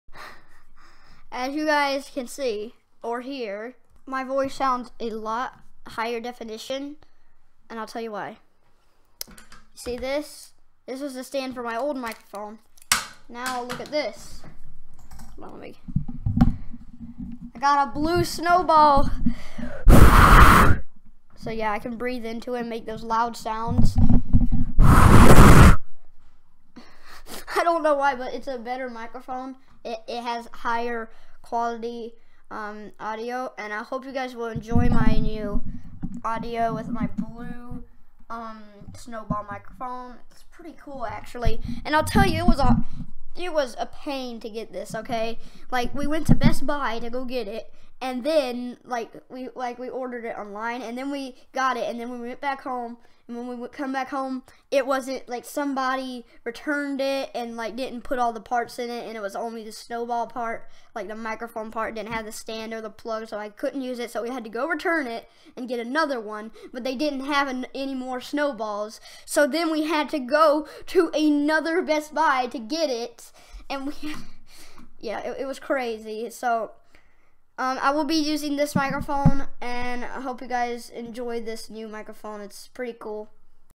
NEW MICROPHONE!
So I got a brand new Blue Snowball microphone that I will be using in my videos from now on!